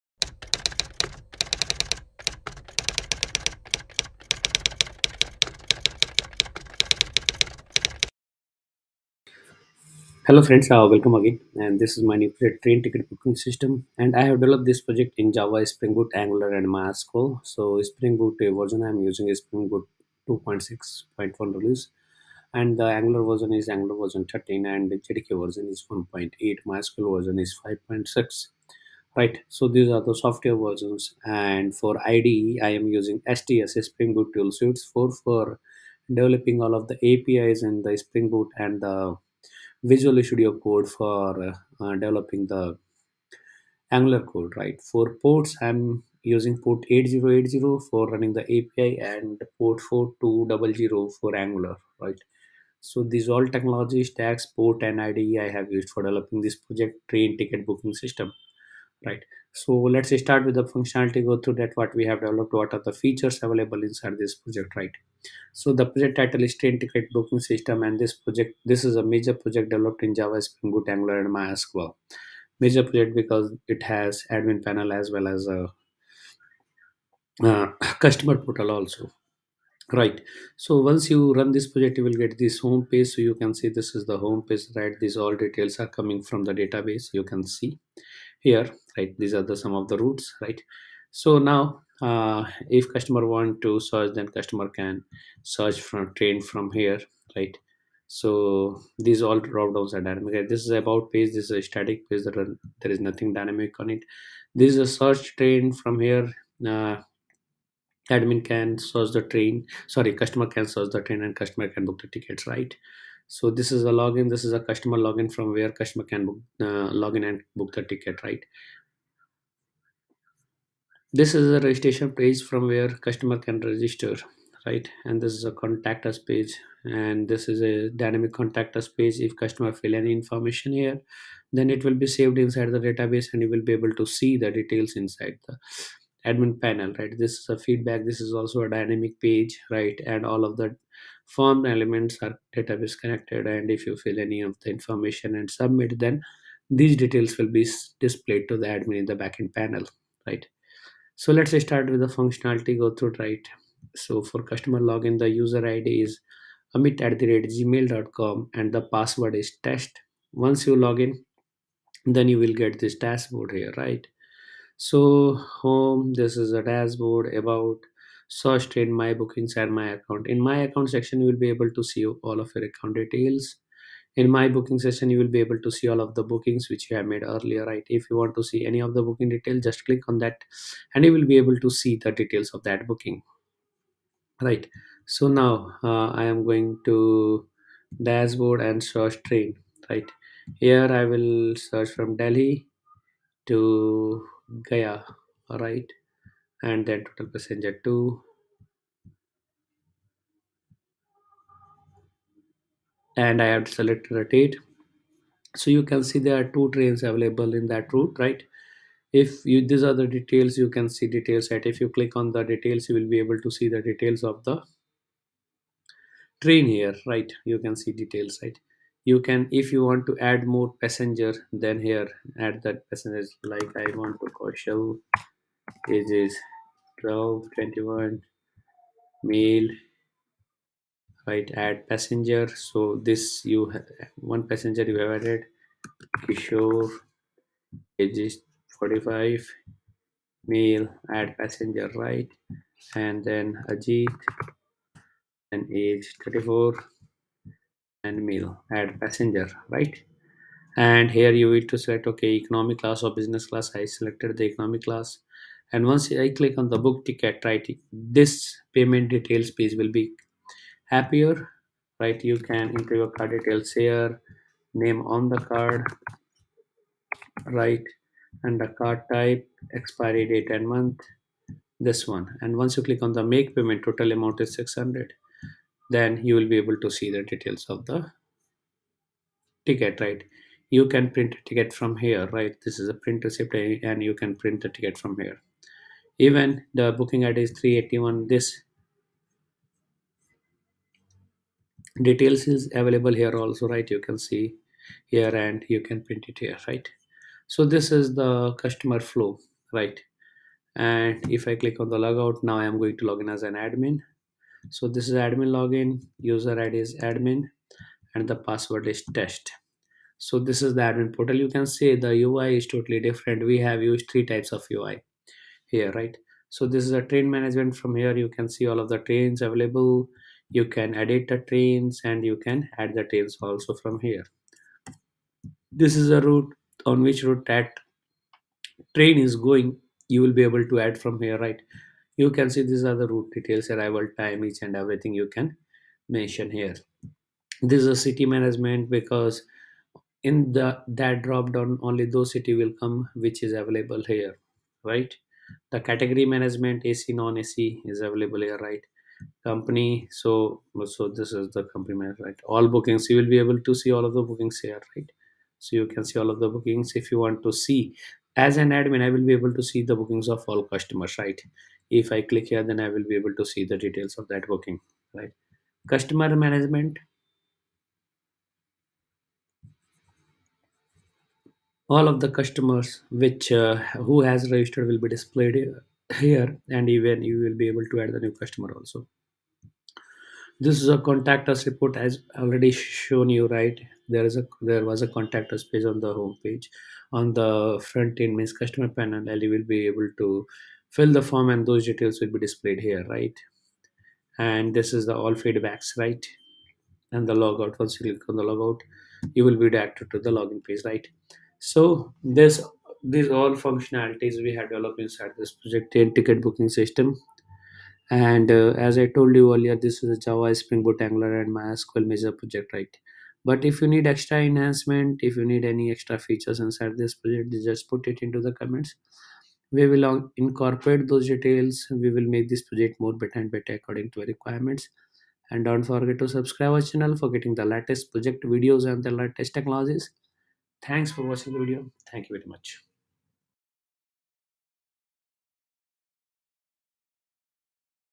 Train Ticket Booking System | Spring Boot Angular Project Tutorial | Spring Boot CRUD Project
Train Ticket Booking System is a spring boot micro service project where all APIs we have developed in spring boot and all UI Train Ticket Booking System  we have implemented in Angular.

Train Ticket Booking System Spring Boot Angular Project Tutorial